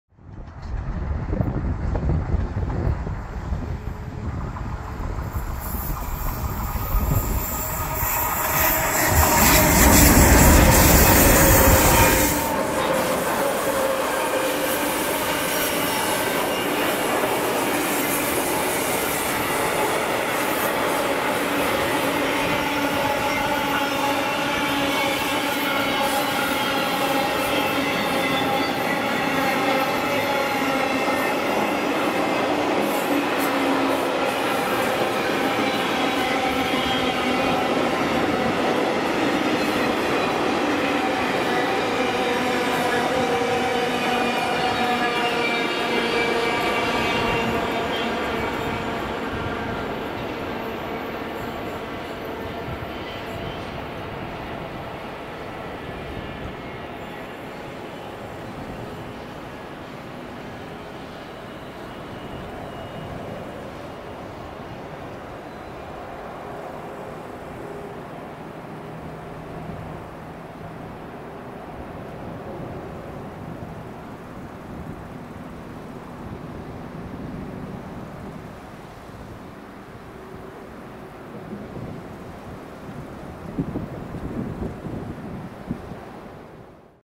Dubbeltractie HLE13 met staaltrein langs de boog van Ekeren!
Een dubbeltractie HLE13 komt door de boog van Ekeren met een staaltrein.

► Social Media!